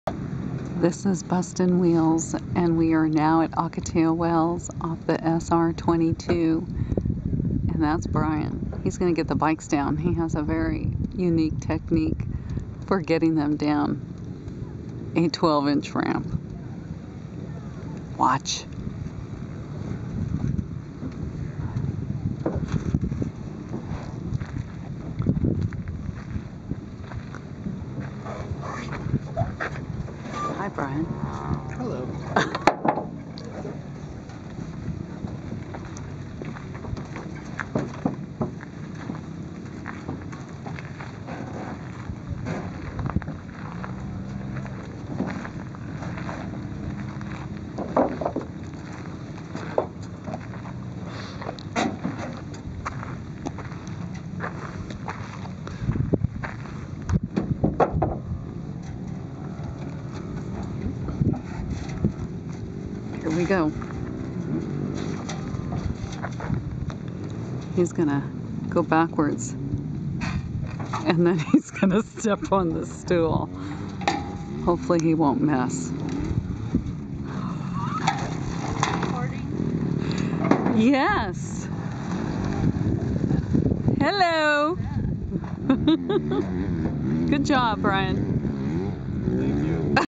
Ocotillo Wells Dirt Biking - Will Brian drop the bike?
Hi - Bustinwheels here video taping Brian unloading the bikes. Yikes!